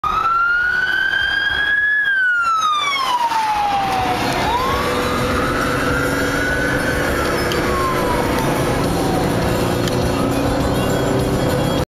Ambulance Responding in Loudon, NH
Speedway Safety ambulance transporting in Loudon in 2011.